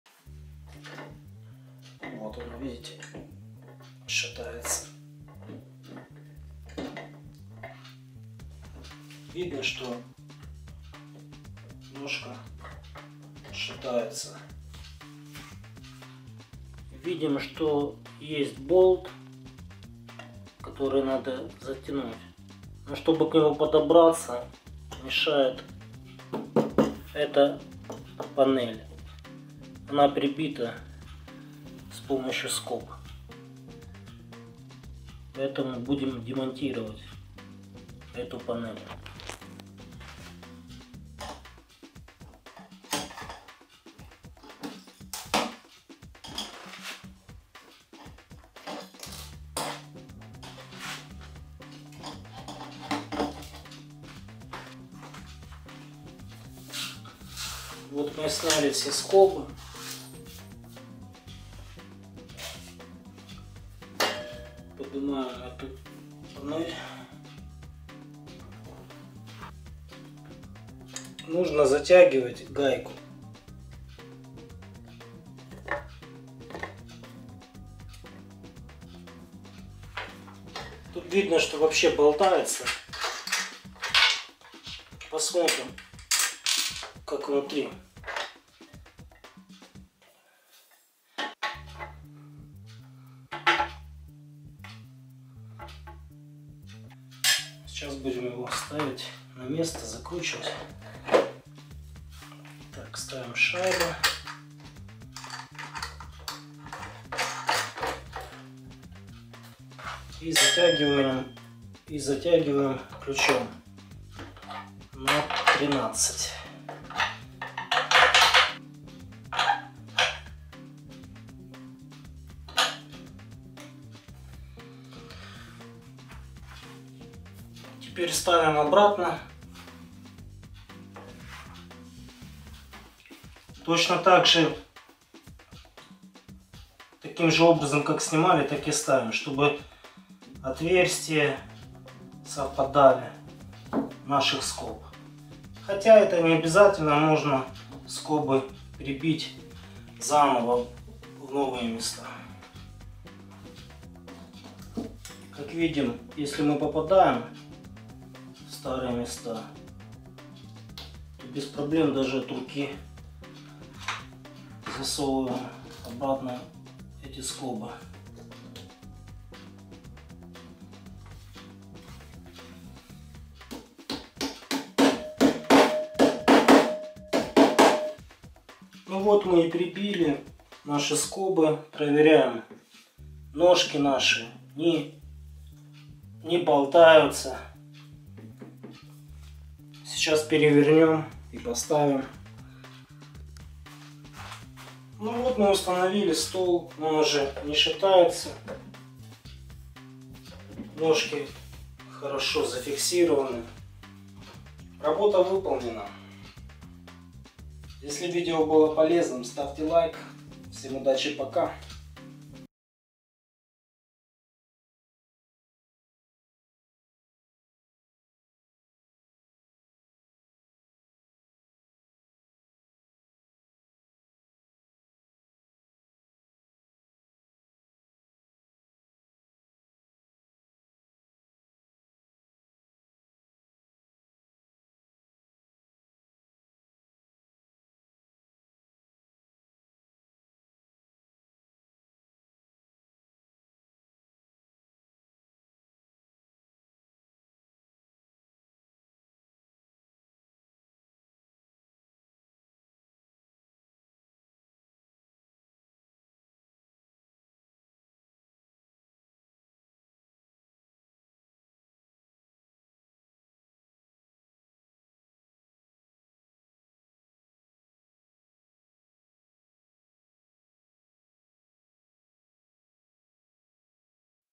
Как укрепить ножки стола если они шатаются. Ремонт кухонного стола. Подтяжка металлических ножек.
Металлические ножки стола шатаются? Тогда стол подлежит ремонту. В видео показано, как затянуть винт ножки стола.